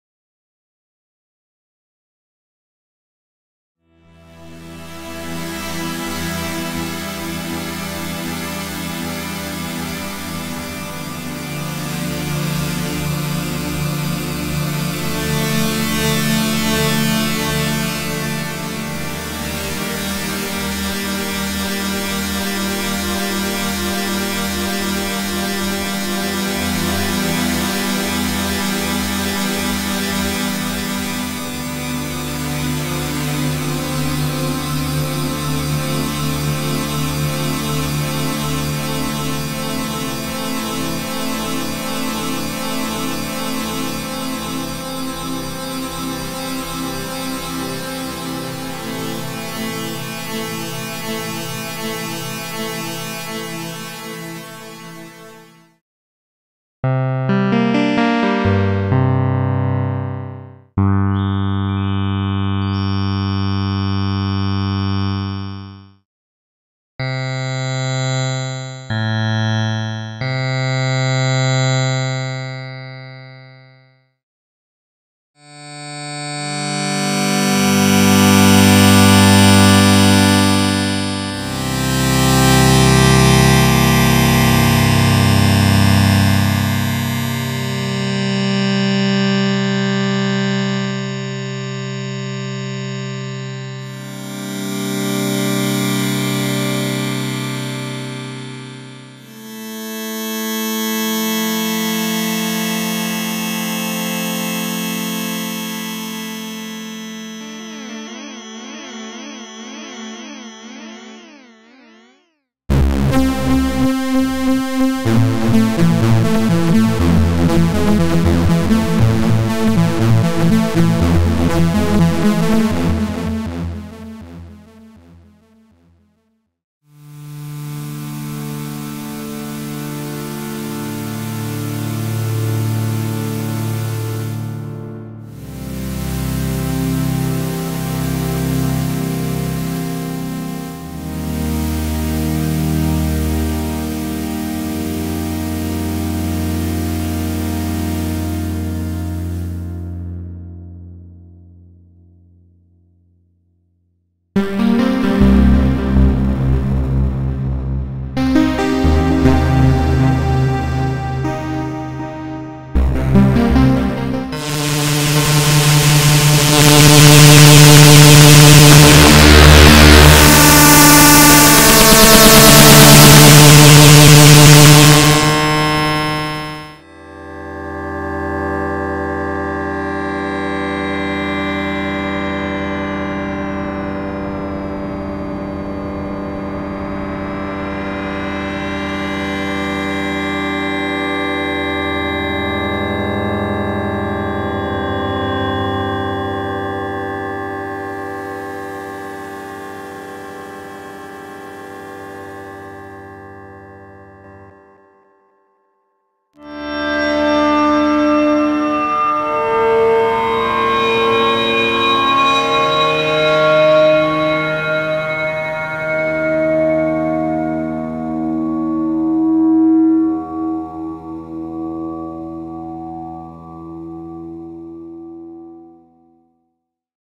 Phosphor 2 iPad Demo
Demo of Phosphor 2 by Audio Damage
played on CME XKey Air bluetooth midi keyboard